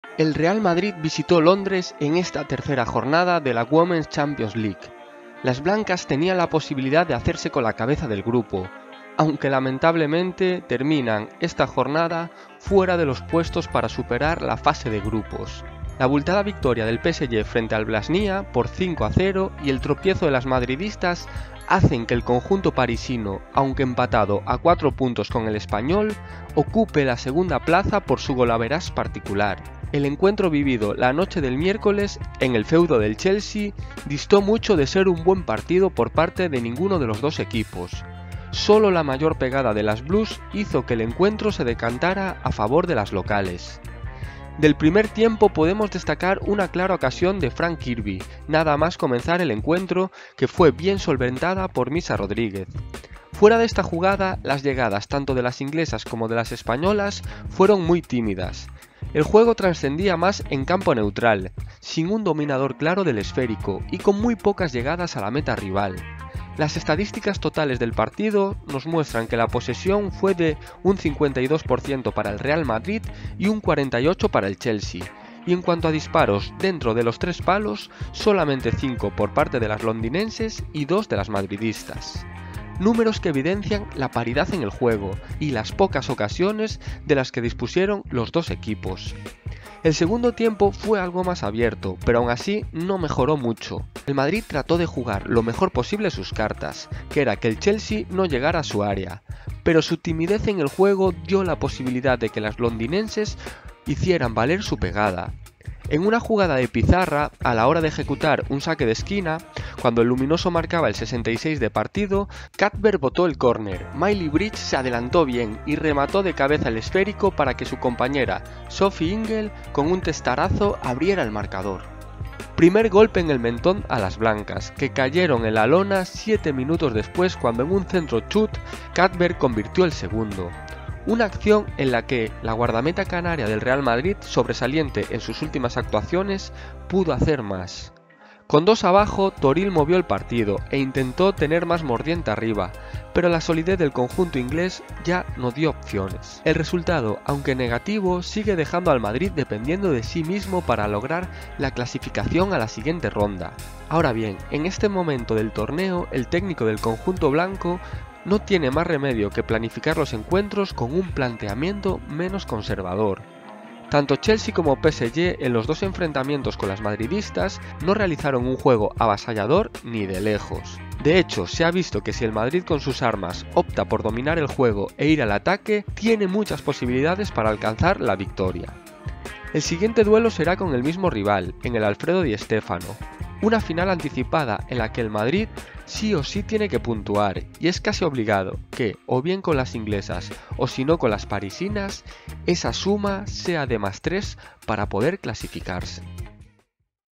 Crónica CHELSEA women vs REAL MADRID femenino. Dura derrota en Londres. (WOMEN´S CHAMPIONS LEAGUE)
Crónica jornada 3 Women´s Champions League. El Chelsea se impone al Real Madrid. Las blancas se complican el pase.